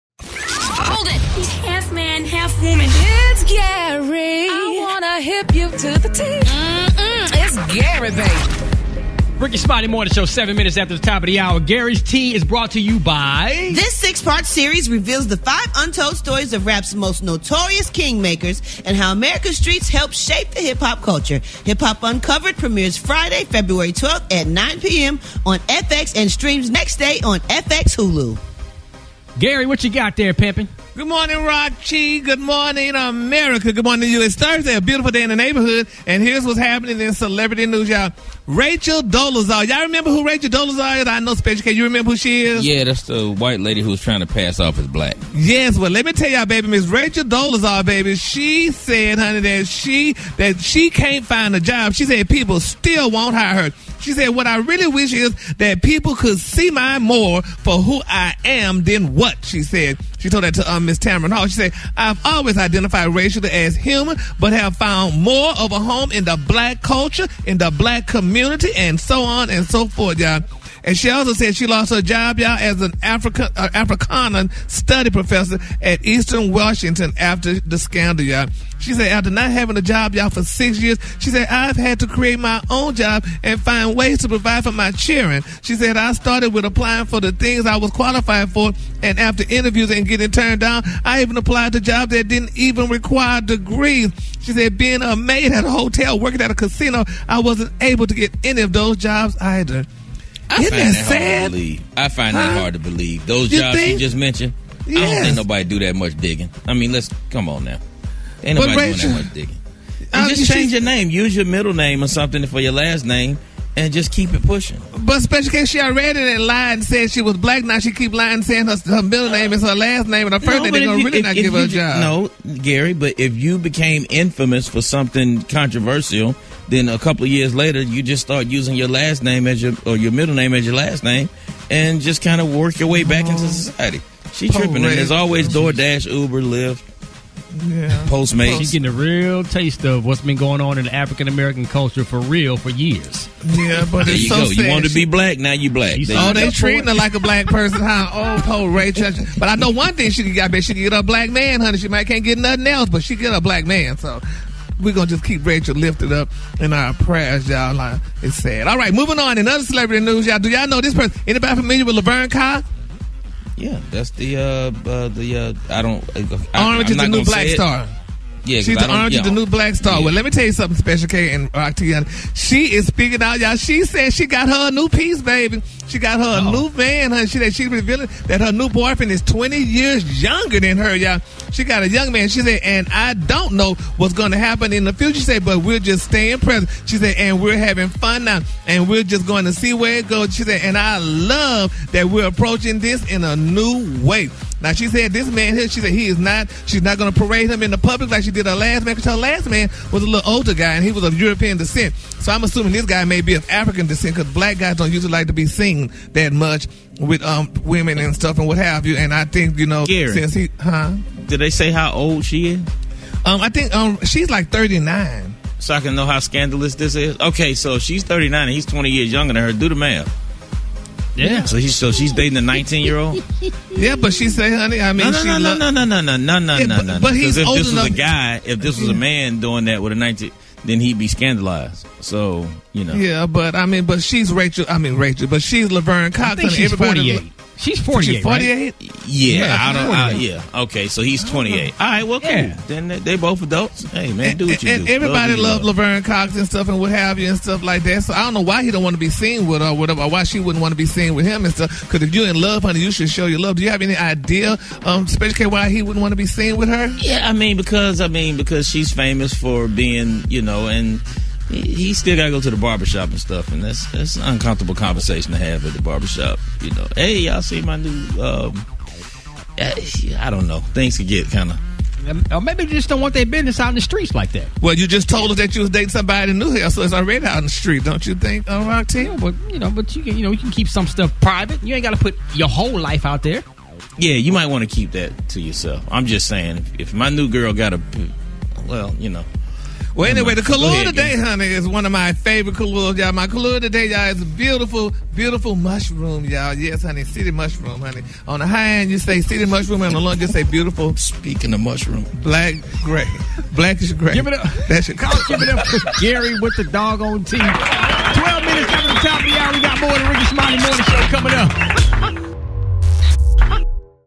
Rachel Dolezal's Struggle | Laverne Cox's New Man
I have the Tea on Rachel Dolezal struggling to find a job after the scandal that made her famous. And in other celebrity news, Laverne Cox has a new man who's much younger, but she's keeping him out of the spotlight. GaryWithDaTea.com